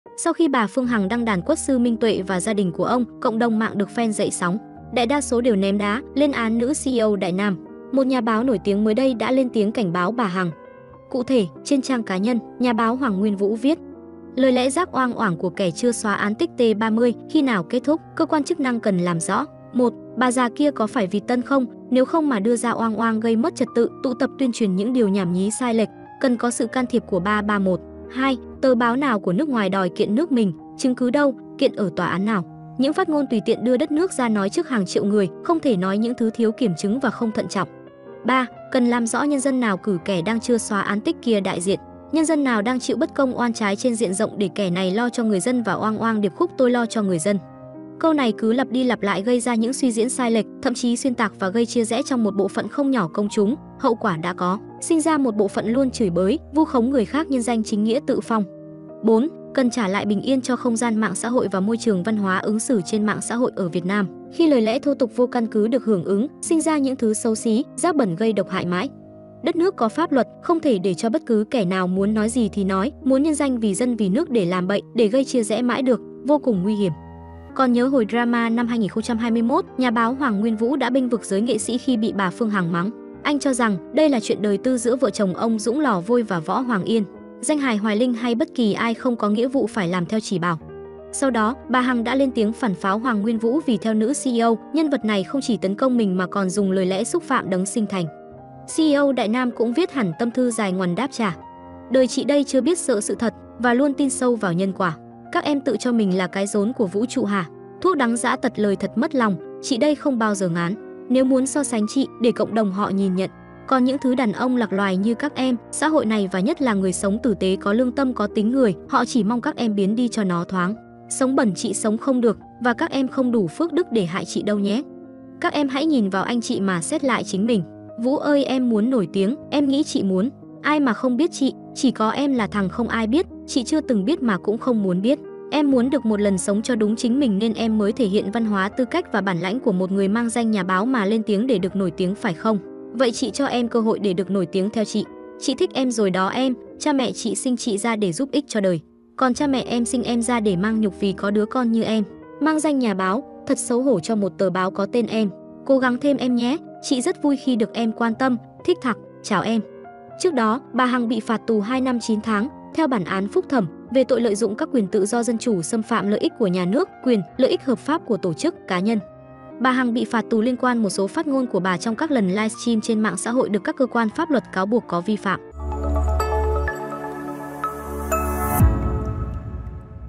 Hoàng Nguyên Vũ cảnh cáo bà Phương Hằng, cấm 1 thứ
Sau khi bà Phương Hằng đăng đàn quất sư Minh Tuệ và gia đình của ông, cộng đồng mạng được phen dậy sóng. Đại đa số đều ném đá, lên án nữ CEO Đại Nam. Một nhà báo nổi tiếng mới đây đã lên tiếng cảnh báo bà Hằng.

Còn nhớ hồi drama năm 2021, nhà báo Hoàng Nguyên Vũ đã bênh vực giới nghệ sĩ khi bị bà Phương Hằng mắng. Anh cho rằng, đây là chuyện đời tư giữa vợ chồng ông Dũng lò vôi và Võ Hoàng Yên. Danh hài Hoài Linh hay bất kì ai không có nghĩa vụ phải làm theo chỉ bảo.

Sau đó, bà Hằng đã lên tiếng phản pháo Hoàng Nguyên Vũ vì theo nữ CEO, nhân vật này không chỉ tấn công mình mà còn dùng lời lẽ xúc phạm đấng sinh thành.